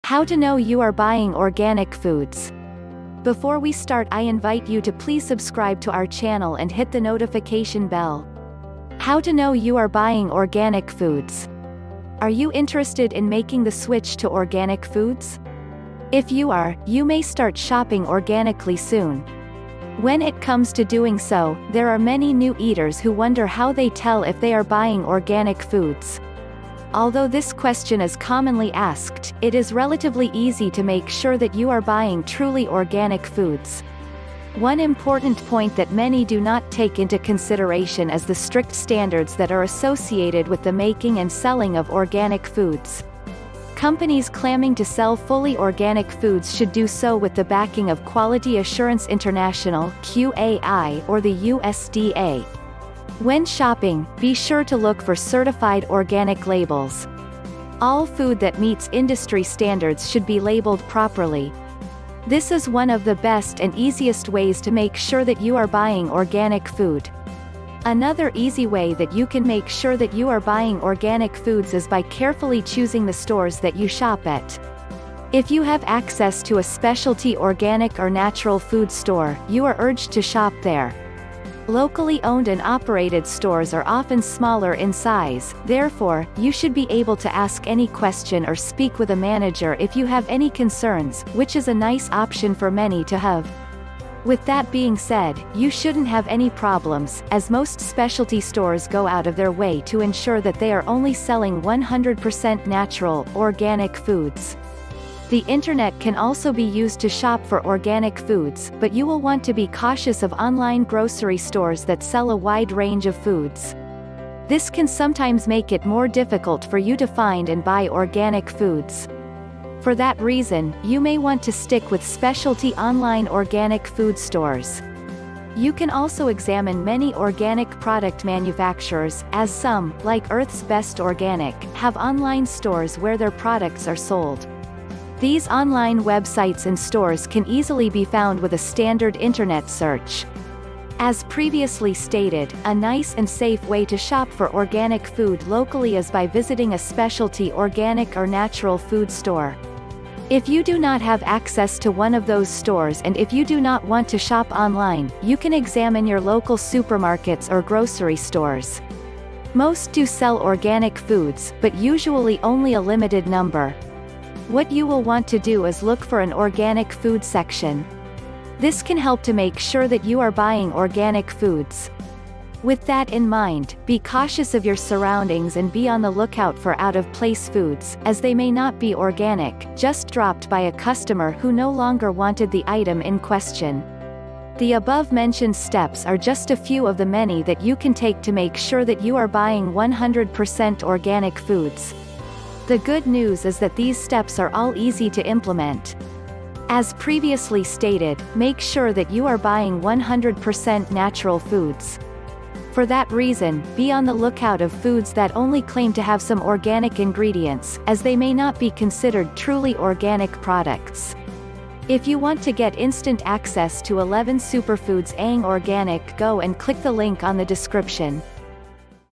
How To Know You Are Buying Organic Foods?
Are you interested in making the switch to organic foods?  If you are, you may start shopping organically soon.  When it comes to doing so, there are many new eaters who wonder how they tell if they are buying organic foods.  Although this question is commonly asked, it is relatively easy to make sure that you are buying truly organic foods.

One important point that many do not take into consideration is the strict standards that are associated with the making and selling of organic foods.  Companies claming to sell fully organic foods should do so with the backing of Quality Assurance International (QAI) or the USDA.  When shopping, be sure to look for certified organic labels.  All food that meets industry standards should be labeled properly. This is one of the best and easiest ways to make sure that you are buying organic food.

Another easy way that you can make sure that you are buying organic foods is by carefully choosing the stores that you shop at.  If you have access to a specialty organic or natural food store, you are urged to shop there.  Locally owned and operated stores are often smaller in size; therefore, you should be able to ask any question or speak with a manager if you have any concerns, which is a nice option for many to have.  With that being said, you shouldn’t have any problems, as most specialty stores go out of their way to ensure that they are only selling one hundred percent natural, organic foods.

The internet can also be used to shop for organic foods, but you will want to be cautious of online grocery stores that sell a wide range of foods.  This can sometimes make it more difficult for you to find and buy organic foods.  For that reason, you may want to stick with specialty online organic food stores.  You can also examine many organic product manufacturers, as some, like Earth’s Best Organic, have online stores where their products are sold.  These online websites and stores can easily be found with a standard internet search.

As previously stated, a nice and safe way to shop for organic food locally is by visiting a specialty organic or natural food store.  If you do not have access to one of those stores and if you do not want to shop online, you can examine your local supermarkets or grocery stores.  Most do sell organic foods, but usually only a limited number.  What you will want to do is look for an organic food section.  This can help to make sure that you are buying organic foods.  With that in mind, be cautious of your surroundings and be on the lookout for out of place foods, as they may not be organic, just dropped by a customer who no longer wanted the item in question.

The above mentioned steps are just a few of the many that you can take to make sure that you are buying one hundred percent organic foods.  The good news is that these steps are all easy to implement.  As previously stated, make sure that you are buying one hundred percent natural foods.  For that reason, be on the lookout of foods that only claim to have some organic ingredients, as they may not be considered truly organic products.